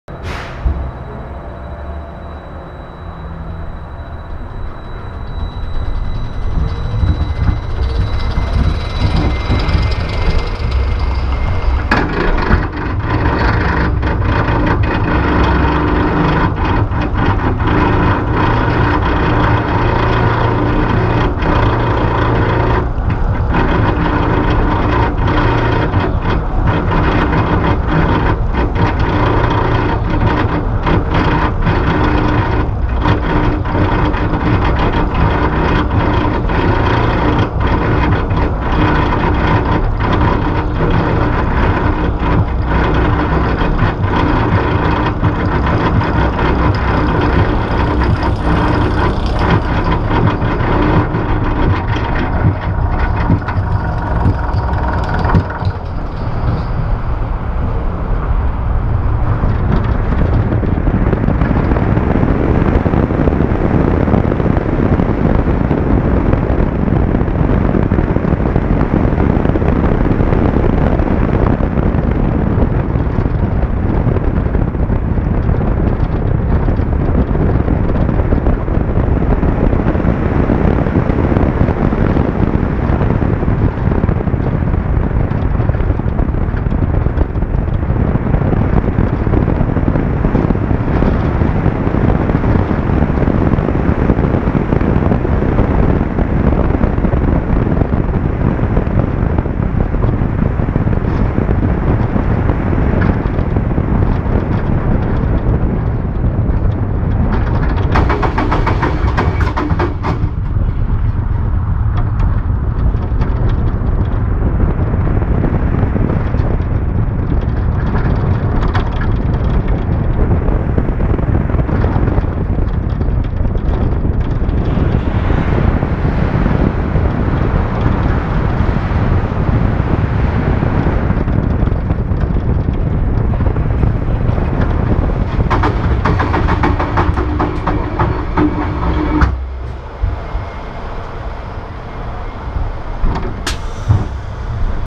Wild Thing POV
Valleyfair's hyper-coaster, features one of the world's largest weightless zones and a 200-foot, 60-degree drop! The tallest and fastest roller coaster at Valleyfair, standing 207 feet at its highest point with a first drop of 200 feet at a 60-degree angle and reaching speeds up to 74 miles per hour. Wild Thing features four drops of more than 100 feet along its 5,460-foot-long course (207', 103', 130', 103'). Out and back steel coaster with two "S" curves, one tunnel and a 360-degree spiral. Longest low-gravity section of any coaster in the world.